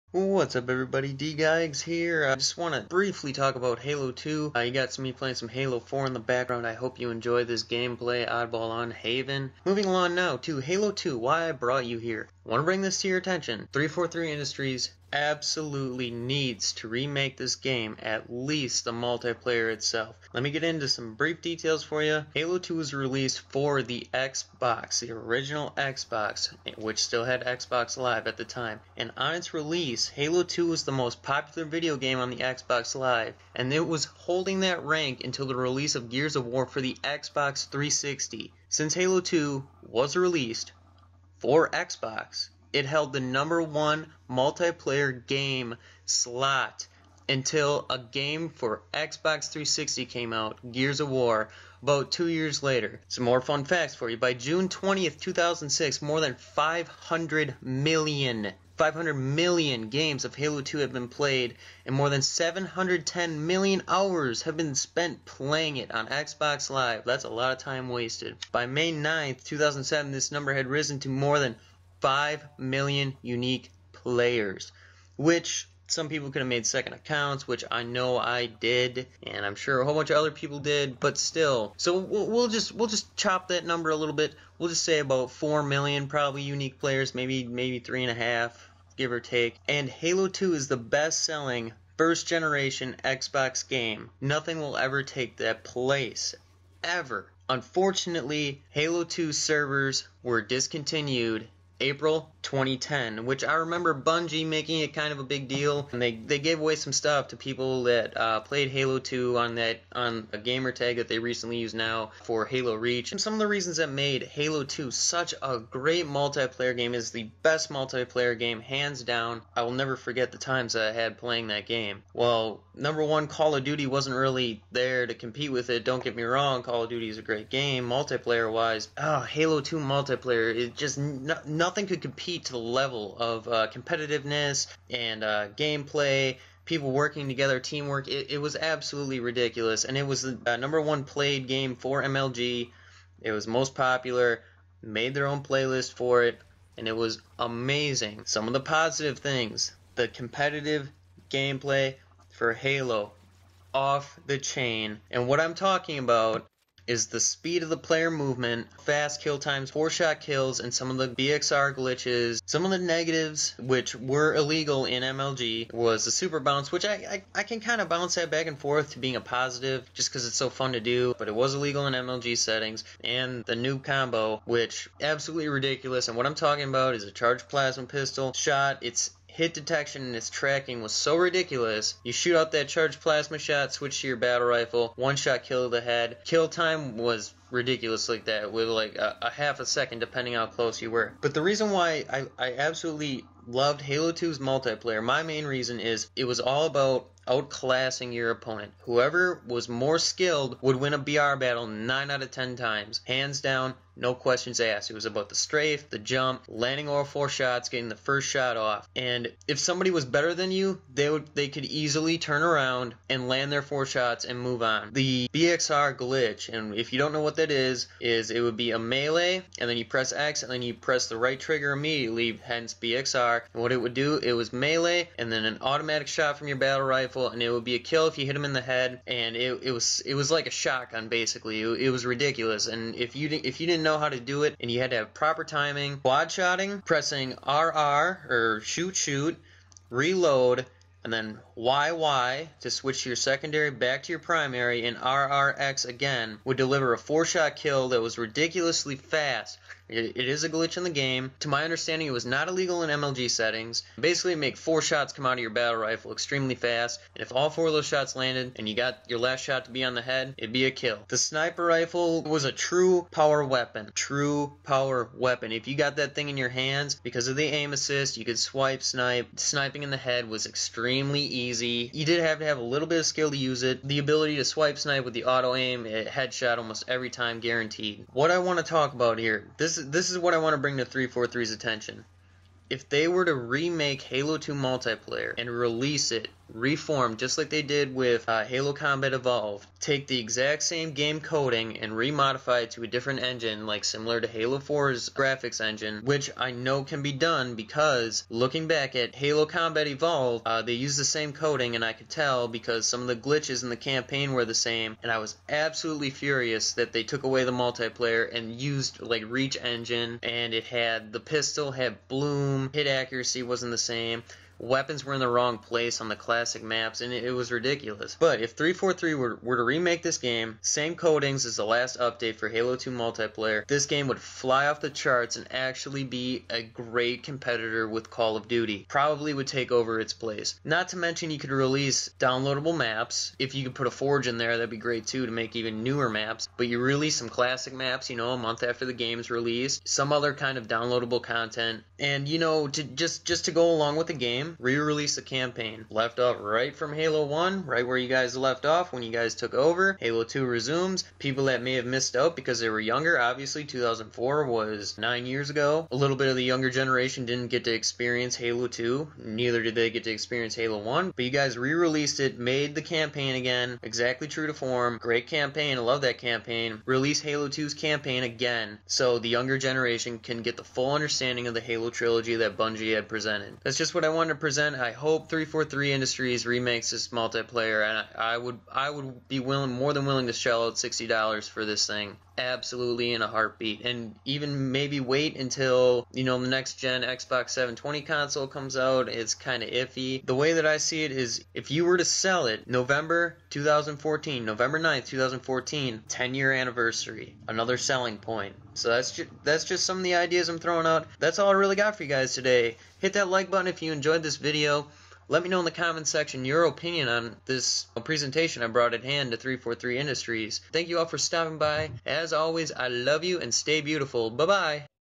Remake Halo 2 Multiplayer?
Just briefly describing why I think Halo 2 should be remade. Hope you enjoy.

Halo 2
Halo 4
Halo Multiplayer